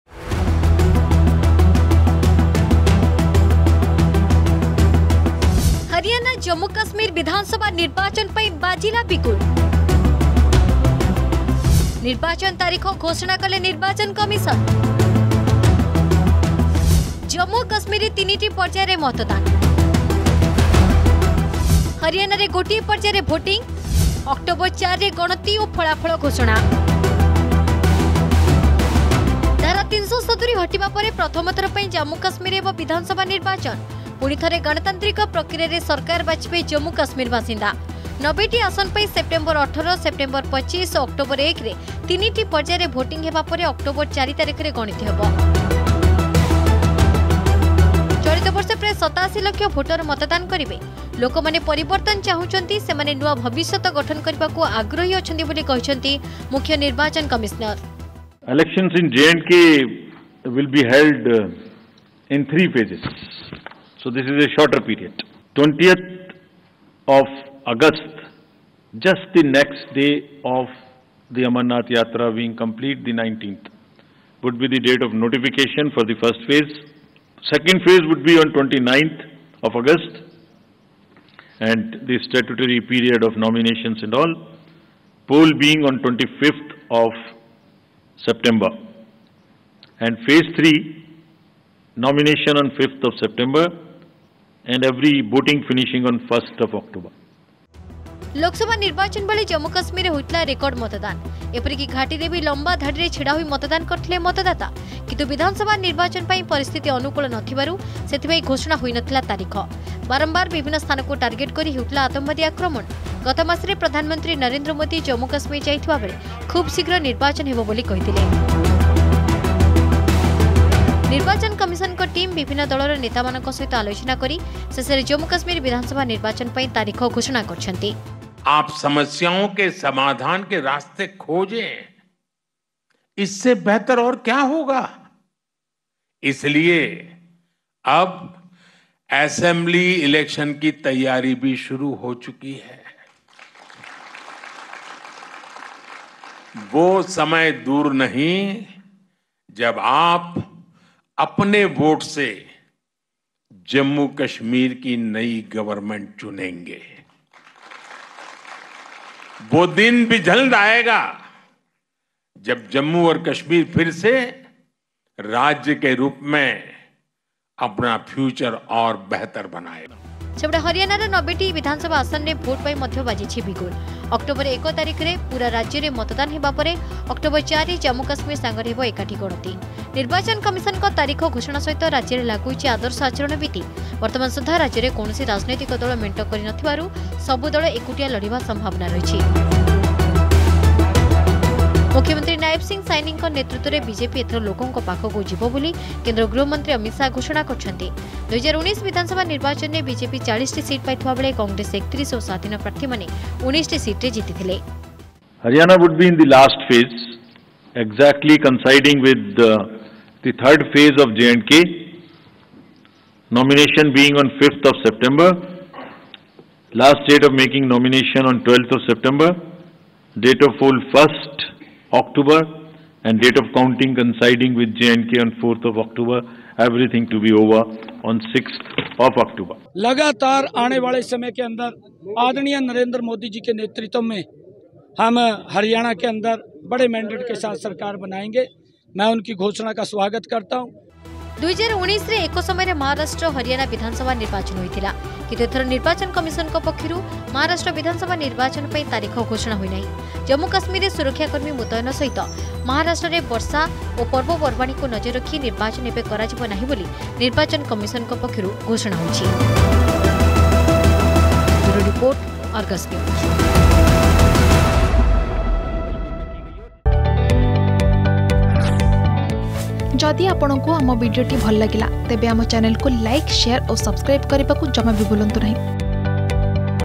ହରିଆଣା, ଜମ୍ମୁ ଓ କାଶ୍ମୀର ବିଧାନସଭା ନିର୍ବାଚନ ପାଇଁ ବାଜିଲା ବିଗୁଲ || Election 2024
ହରିୟାଣା, ଜମ୍ମୁ ଓ କାଶ୍ମୀର ବିଧାନସଭା ନିର୍ବାଚନ ବିଗୁଲ ବାଜିଛି। ନିର୍ବାଚନ କମିଶନ ଆଜି ତାରିଖ ଘୋଷଣା କରିଛନ୍ତି। ଜମ୍ମୁ ଓ କାଶ୍ମୀରର 90ଟି ବିଧାନସଭା ଆସନ ପାଇଁ ତିନିଟି ପର୍ଯ୍ୟାୟରେ, ସେପ୍ଟେମ୍ବର ୧୮, ସେପ୍ଟେମ୍ବର ୨୫ ଓ ଅକ୍ଟୋବର ୧ରେ ଭୋଟିଂ ହେବ।